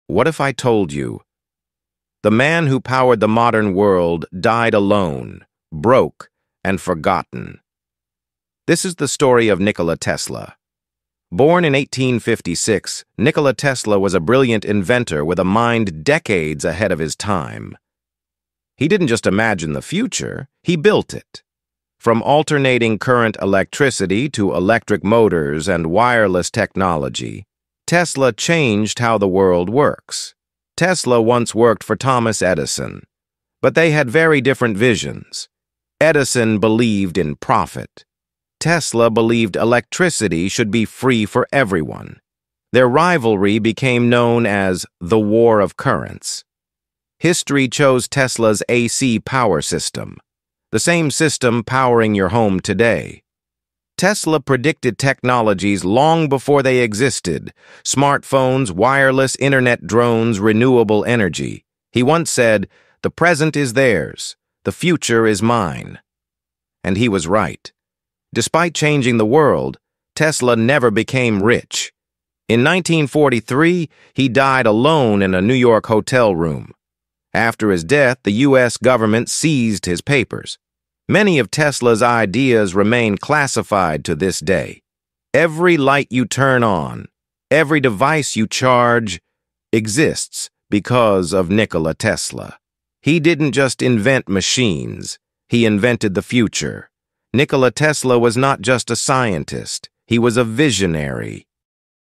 Nikola Tesla: The Man Who Invented the Future (Genius They Tried to Forget)
Nikola Tesla was one of the greatest minds in human history — yet he died alone, broke, and misunderstood.

From alternating current electricity to wireless technology, Tesla’s ideas power the modern world. His rivalry with Thomas Edison, his futuristic predictions, and the mystery surrounding his classified inventions make his story more relevant today than ever.

This cinematic short documentary reveals the truth behind the man who invented the future.

⚡ Watch till the end — the final reveal will change how you see technology forever.